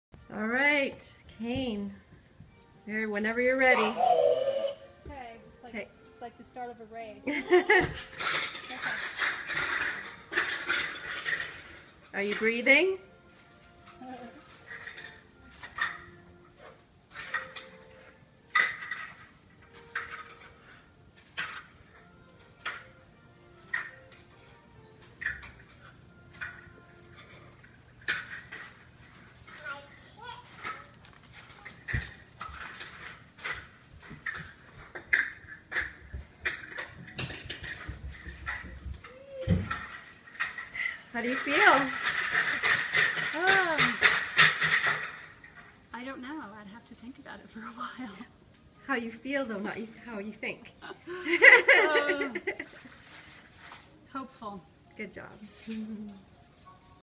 Week 6: Demo of Straight Cane Ambulation Post Hip Arthroscopy
Dr. Ginger Garner, DPT, gets the patient perspective and provides a front row seat in her journey through hip preservation surgery (arthroscopy). Week 6: Demo of Straight Cane Ambulation Post Hip Arthroscopy.